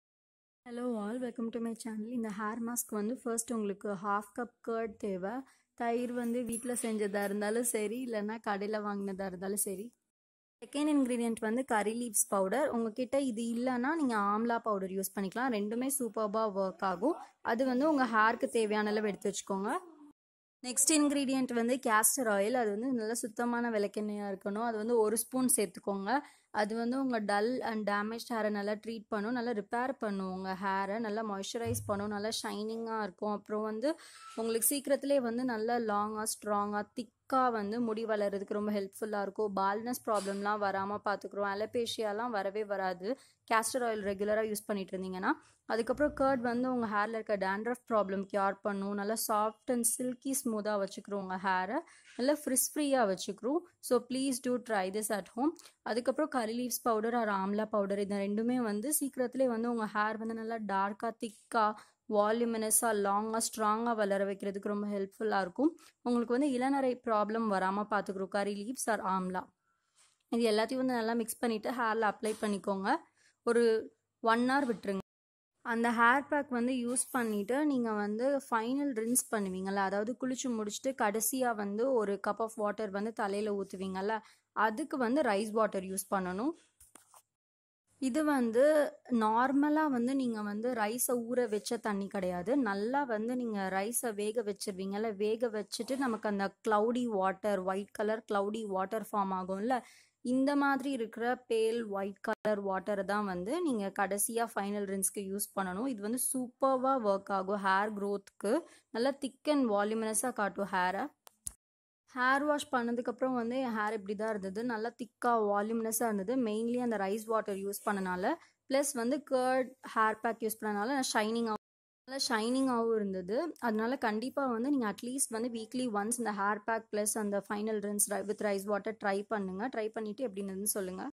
Faster hair growth formula with 2 steps/Hair pack & Rice water hack for thick & voluminous hair
Following these two simple steps at least once in a week will definitely give you awesome results in Just 2 months.This is such an amazing faster hair growth formula with 2 simple steps .Get long/strong/dark/thick and voluminous hair within few uses 💞